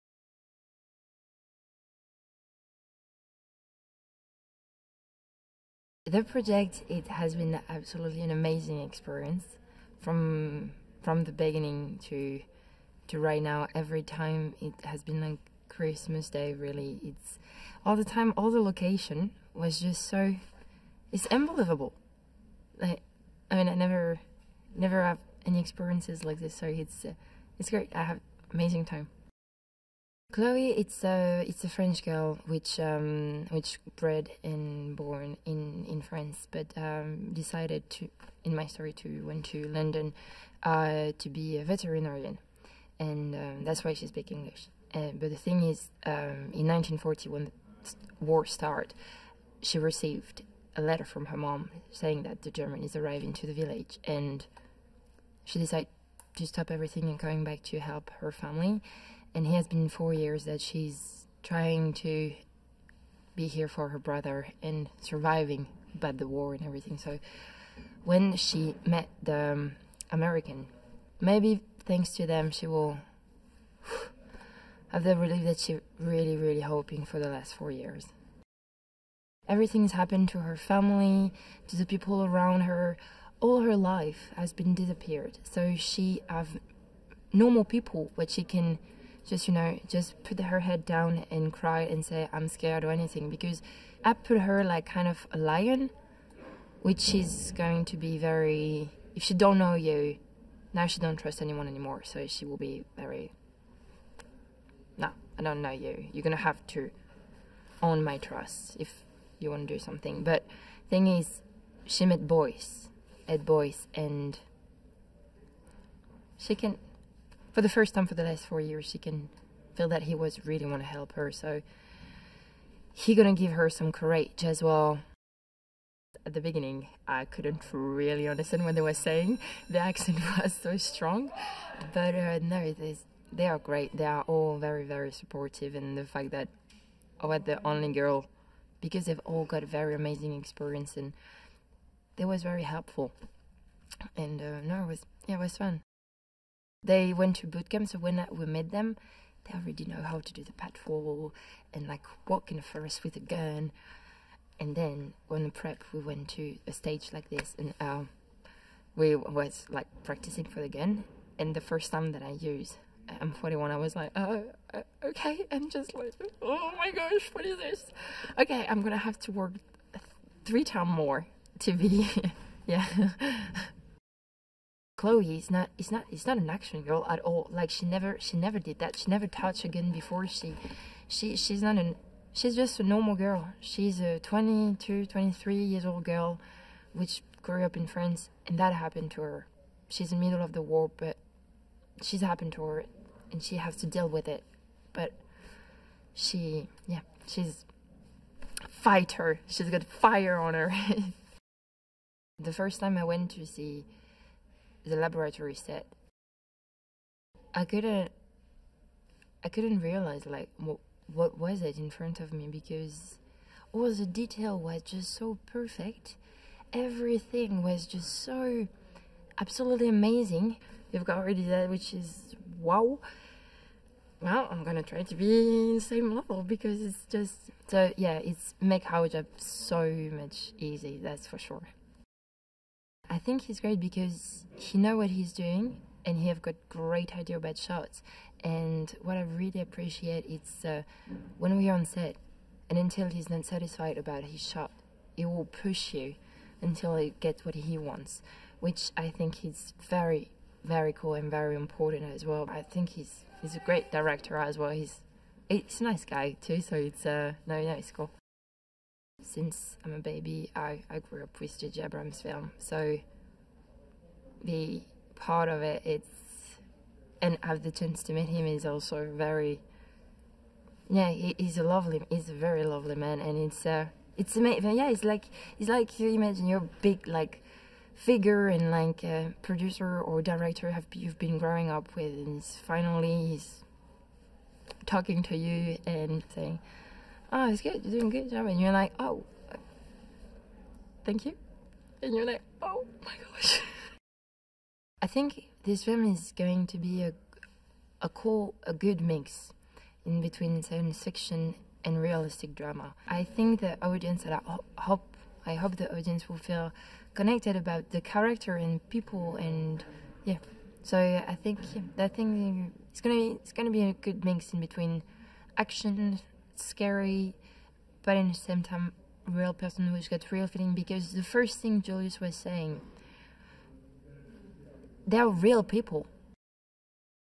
Overlord - Itw Mathilde Ollivier (official video)
Overlord is a 2018 American war horror film directed by Julius Avery and written by Billy Ray and Mark L. Smith. The film stars Jovan Adepo, Wyatt Russell, Jacob Anderson, Dominic Applewhite, Pilou Asbæk, Iain De Caestecker, John Magaro, Mathilde Ollivier and Bokeem Woodbine. J. J. Abrams serves as a producer through his Bad Robot Productions banner with Lindsey Weber. The plot follows several American soldiers who get stuck behind enemy lines after D-Day and discover secret Nazi experiments.Overlord is scheduled to be released in the United States on November 9, 2018, by Paramount Pictures.

Synopsis :
On the eve of D-Day during World War II, American paratroopers are caught behind enemy lines after their plane crashes on a mission to destroy a German Radio Tower in a small town outside of Normandy. After reaching their target, the paratroopers come to realize that besides fighting off Nazi soldiers, they also must face off against horrifying, bloody, and violent creatures that are a result of a secret Nazi experiment.

Overlord
Directed by Julius Avery
Produced by J. J. Abrams, Lindsey Weber
Screenplay by Billy Ray, Mark L. Smith
Story by Billy Ray
Starring Jovan Adepo, Wyatt Russell, Jacob Anderson, Dominic Applewhite, Pilou Asbæk, Iain De Caestecker, John Magaro, Mathilde Ollivier, Bokeem Woodbine
Cinematography : Laurie Rose, Fabian Wagner
Edited by Matt Evans
Production companies : Paramount Pictures, Bad Robot Productions
Release date : September 22, 2018 (Fantastic Fest), November 9, 2018 (United States)

(Source: Wikipedia)